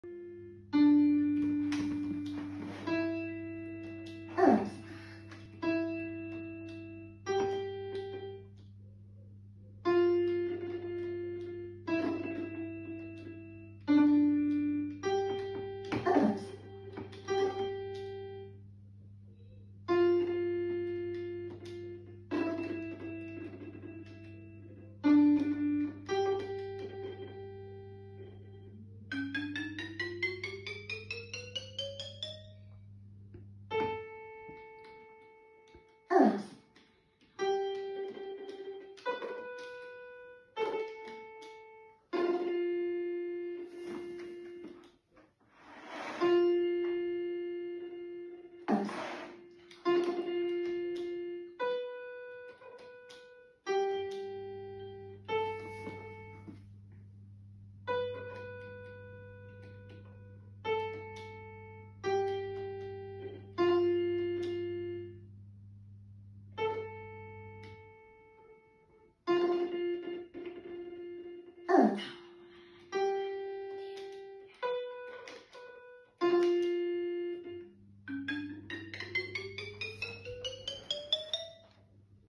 Soft Mozart China: George (7) plays Note alphabet game. Preparatory level C
Teacher: Lara Molaeb